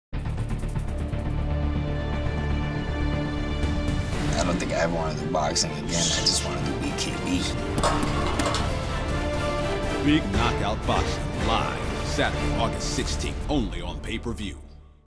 BKB 15s Spot Featuring Javier Garcia
On Sat. Aug. 16 the knockout in boxing will be revitalized at Mandalay Bay.